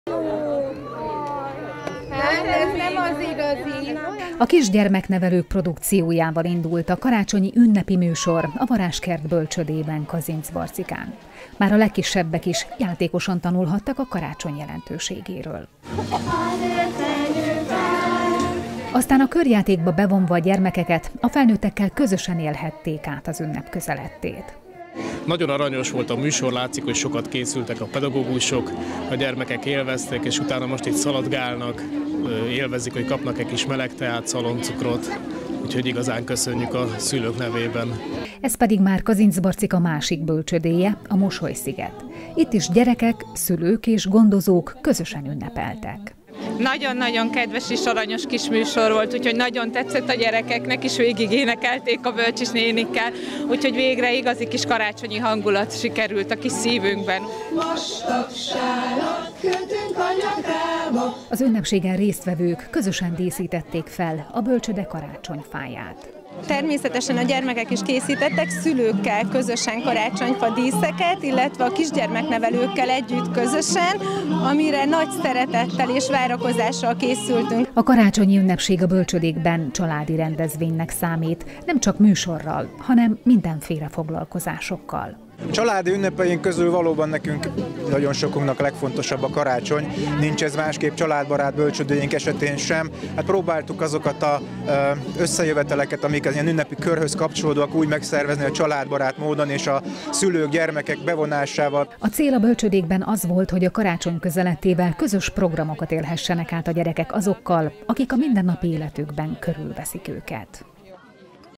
KOLORTV - Bölcsőde karácsony
Jézus Krisztus születésére emlékezik karácsonykor a keresztény világ, a nem keresztények pedig a szeretet ünnepét tartják ekkor. Ez vált mára az öröm, a békesség, a család és az otthon napja. Kazincbarcikán a legkisebbek is ünnepeltek. Erre készültek már napok óta a Varázskert és a Mosolysziget bölcsődékben.